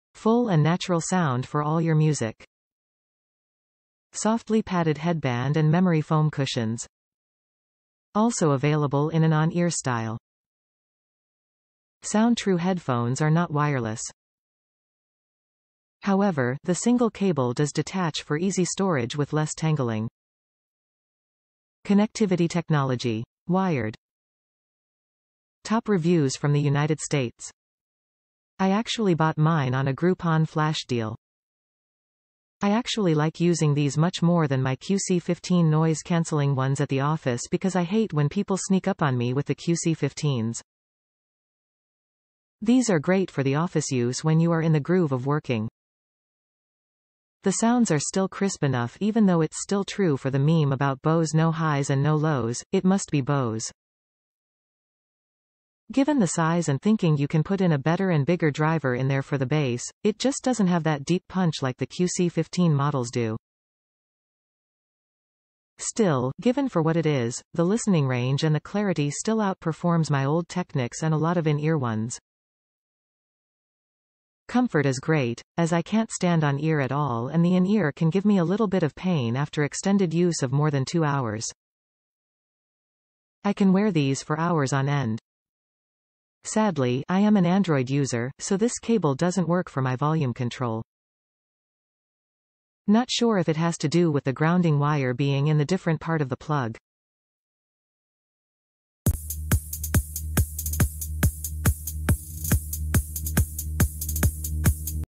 Bose SoundTrue Headphones Around-Ear Style, White (Wired) (Discontinued by Manufacturer) - HIP Shop
Bose SoundTrue Headphones Around-Ear Style, White (Wired) (Discontinued by Manufacturer)

Full and natural sound for all your music
Softly padded headband and memory foam cushions
Also available in an on-ear style
SoundTrue headphones are not wireless. However, the single cable does detach for easy storage with less tangling.
Connectivity technology : Wired

I actually bought mine on a groupon flash deal. I actually like using these much more than my QC15 noise cancelling ones at the office because I hate when people sneak up on me with the QC15s. These are great for the office use when you are in the groove of working. The sounds are still crisp enough even though it's still true for the meme about Bose "no highs and no lows, it must be Bose". Given the size and thinking you can put in a better and bigger driver in there for the bass, it just doesn't have that deep punch like the QC15 models do. Still, given for what it is, the listening range and the clarity still out performs my old Technics and a lot of in-ear ones. Comfort is great (as I can't stand on-ear at all and the in-ear can give me a little bit of pain after extended use of more than 2 hours). I can wear these for hours on end. Sadly, I am an Android user, so this cable doesn't work for my volume control.. not sure if it has to do with the grounding wire being in the different part of the plug: